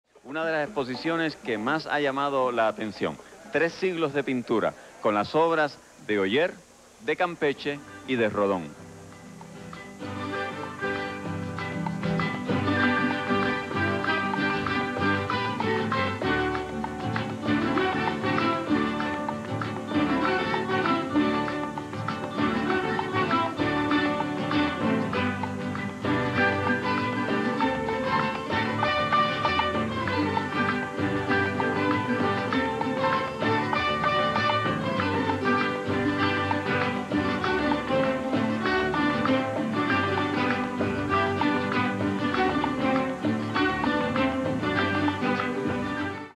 Campeche, Oller y Rodón: Tres Siglos de Pintura Puertorriqueña
Exposición presentada por el Instituto de Cultura Puertorriqueña en el Pabellón de Puerto Rico en la EXPO 92 desde el 23 de junio al 12 de octubre de 1992